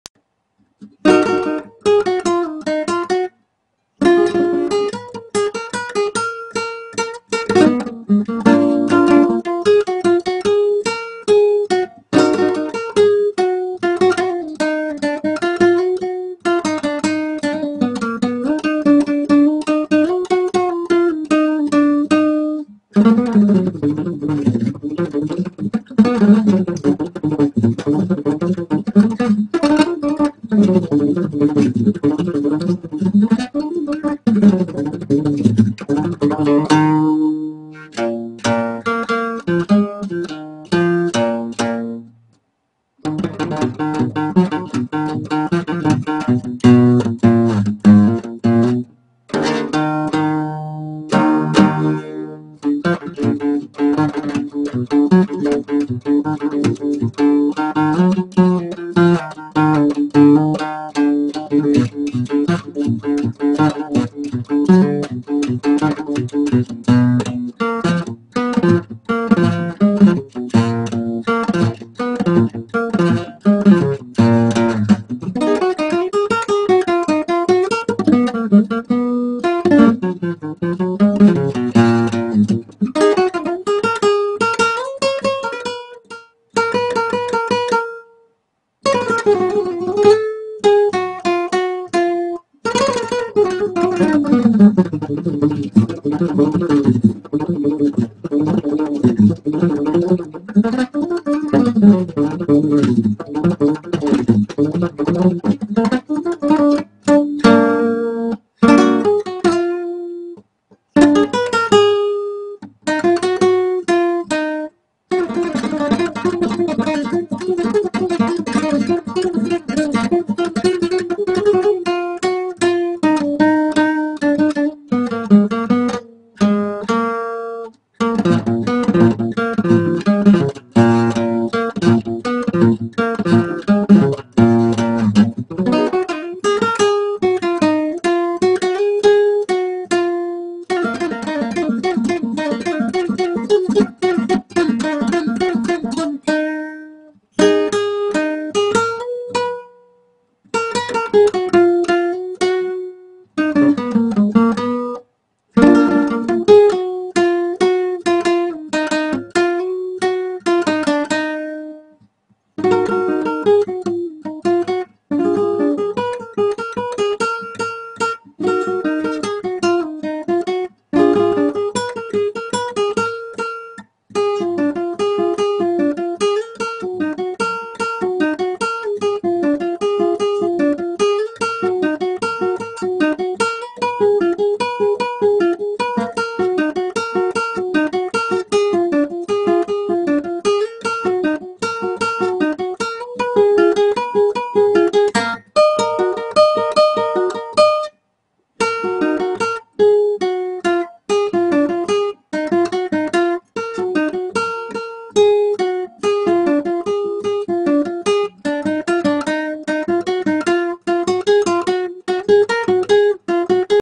Guitarra Dura FlamenCo-lombiana
Improvising Musica vieja Colombiana con un toque Flamenco